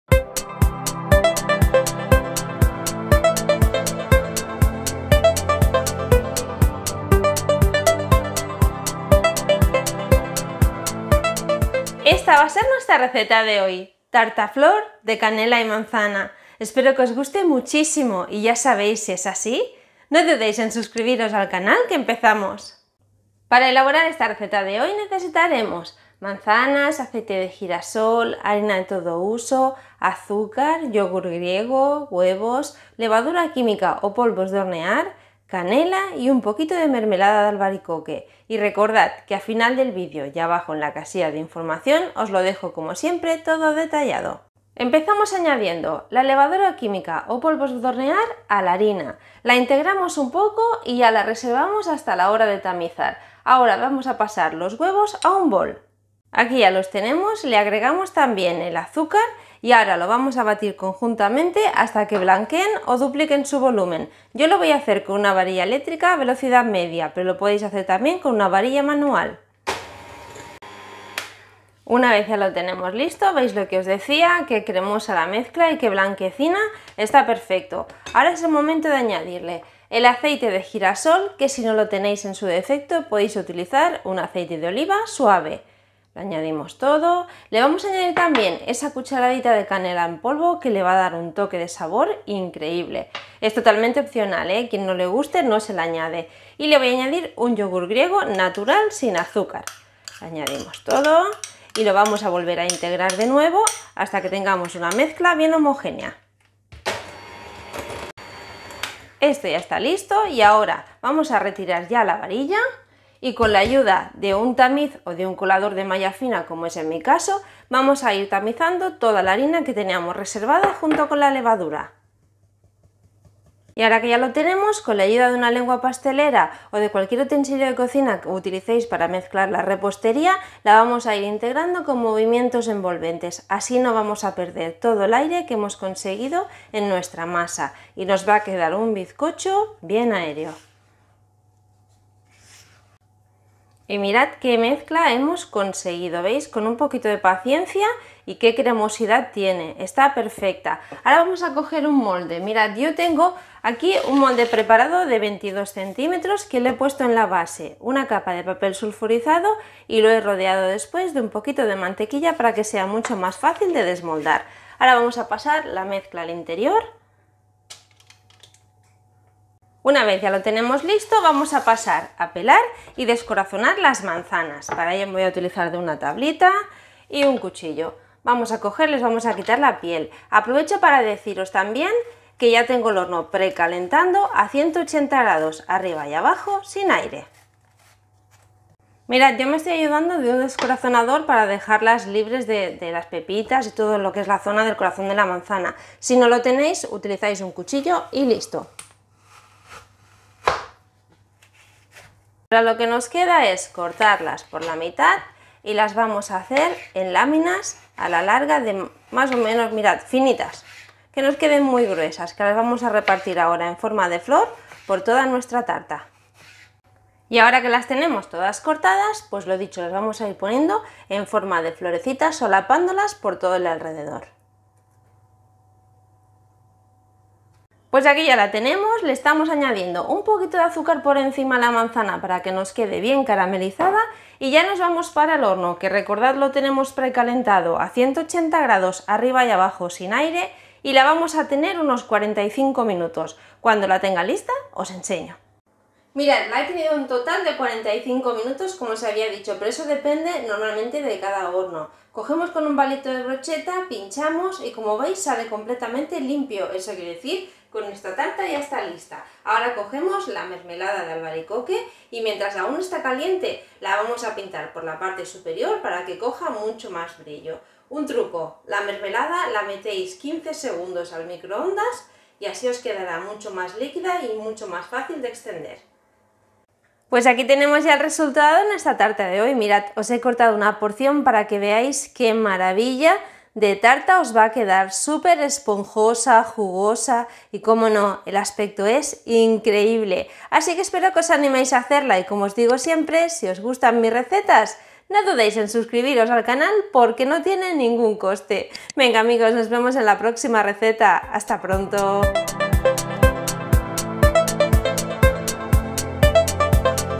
✅️ TARTA FLOR DE CANELA Y MANZANA 🍎 Súper esponjosa y jugosa!
Aquí os traigo una receta muy rica, una tarta flor de canela y manzana que espero que os guste mucho y os animéis a hacerla.
Nos vemos en la próxima receta amigos!

INGREDIENTES
- 3 huevos L
- 1 yogur griego
- 125ml de aceite de girasol
- 235 gr. de azúcar
- 300gr. de harina
- 1 cucharada de canela
- 16 gr. de levadura química
  o polvos de hornear
- 4 manzanas
- mermelada de
  albaricoque